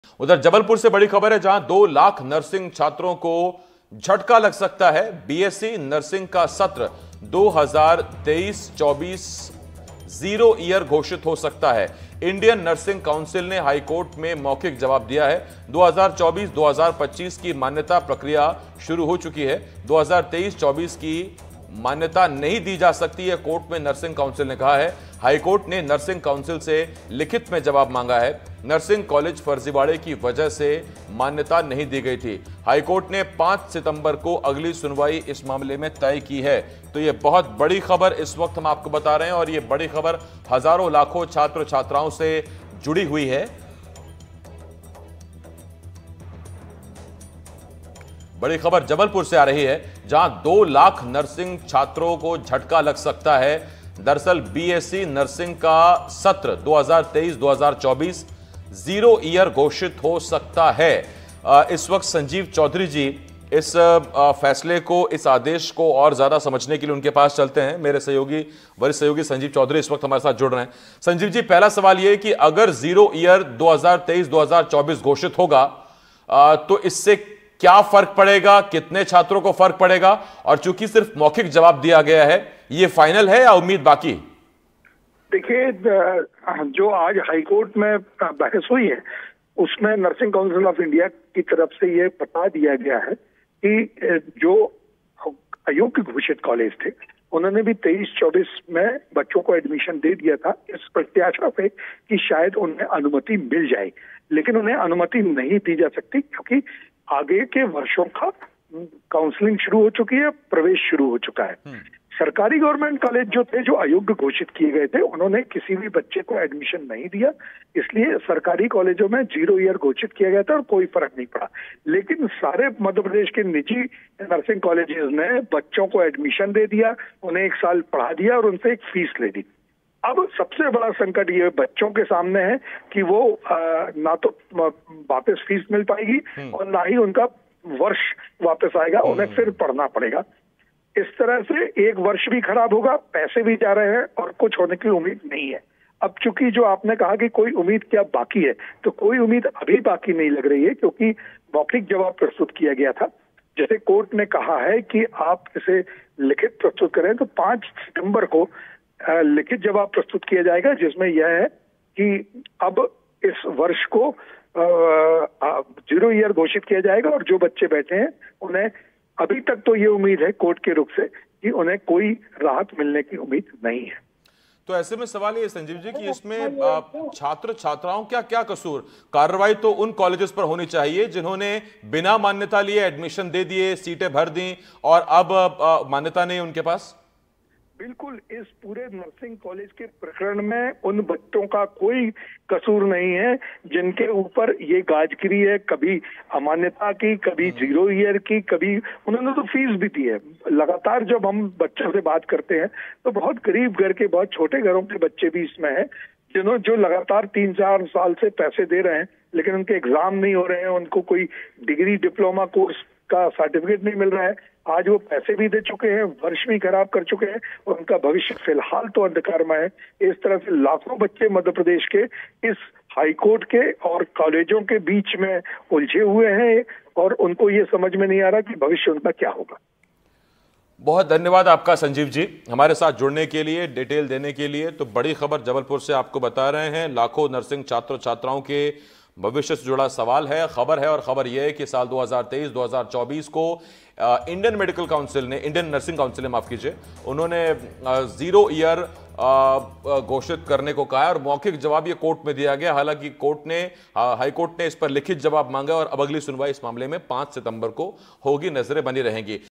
Nursing college fraud: MP के नर्सिंग छात्रों को झटका, 0 Year हो सकता है 2023-24 का सत्र। Breaking
Nursing college fraud: नर्सिंग कॉलेज फर्जीवाड़े (Fraud) की वजह से 2023-24 सत्र के लिए कॉलेजों (College) को मान्यता नहीं दी गई थी. निजी कॉलेजों (College) ने बिना मान्यता सत्र 2023-24 में दाखिले दे दिए थे. HC ने 5 सितंबर को अगली सुनवाई तय की है.